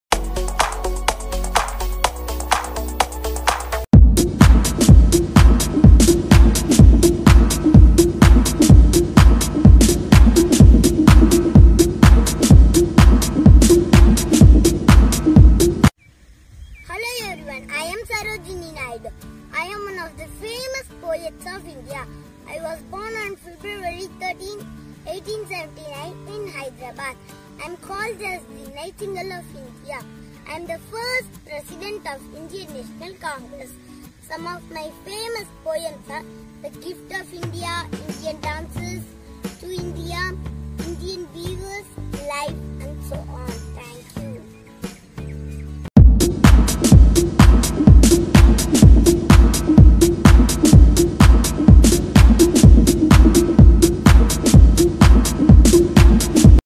Award winning Fancy dress Competition as Sarojini Naidu
This video gives brief information about Sarojini Naidu with appropriate costumes for best fancy dress competition..
This video will be very apt for the kids those who want to enact as Sarojini Naidu.